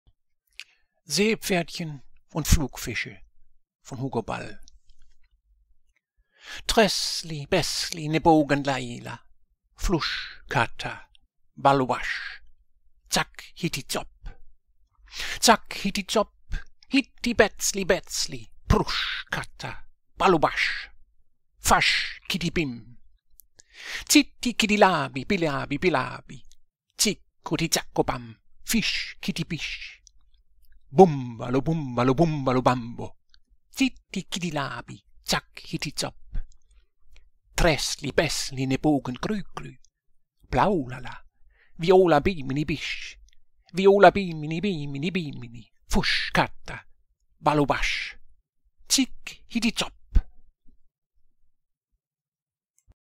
"Seepferdchen und Flugfische" von Hugo Ball, vom Wortmann vorgetragen
Dadaistisches Gedicht.

Seepferdchen und Flugfische

tressli bessli nebogen leila
flusch kata
ballubasch
zack hitti zopp

zack hitti zopp
hitti betzli betzli
prusch kata
ballubasch
fasch kitti bimm

zitti kitillabi billabi billabi
zikko di zakkobam
fisch kitti bisch

bumbalo bumbalo bumbalo bambo
zitti kitillabi
zack hitti zopp

treßli beßli nebogen grügrü
blaulala violabimini bisch
violabimini bimini bimini
fusch kata
ballubasch
zick hiti zopp